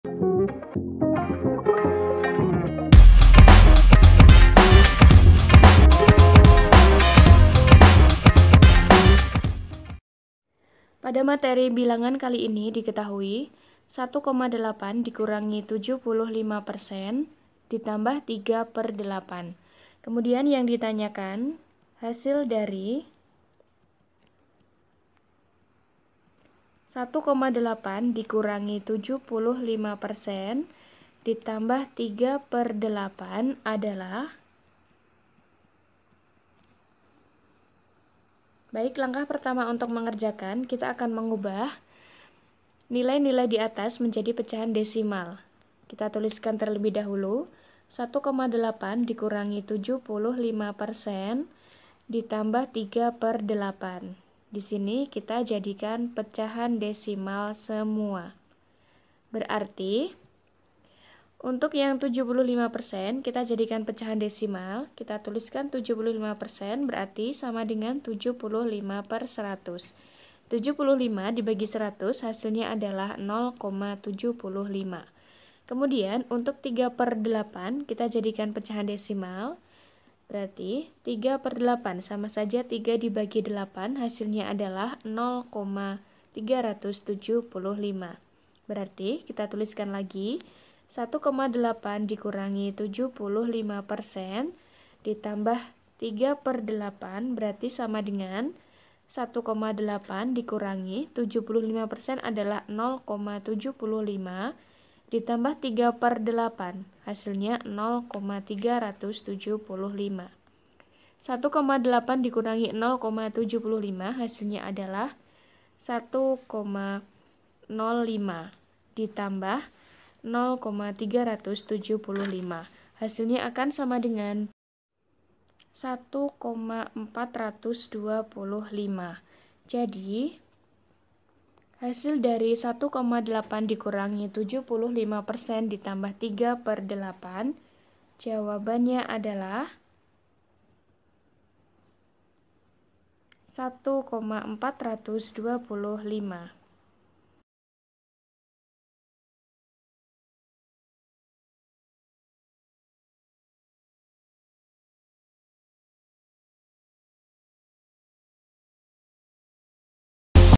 Hasil 1,8-\text{ 75\% +}\frac{3}{8}\text{... | PR Matematika S | Kelas 7
✅ Dalam video ini kita akan membahas: Hasil  1,8-\text{ 75\% +}\frac{3}{8}\text{ adalah }\ldots.
a. 0,675
b. 0,765
c. 1,245
d. 1,425
Tema:
Kelas: 7
Mata Pelajaran: Matematika
Bab: 1
Buku: PR Matematika SMP/MTS Kelas VII Semester 1

👉 Buku yang telah tersedia:
{PLAYLIST_LINK}
Di mana kamu dapat menemukan kami?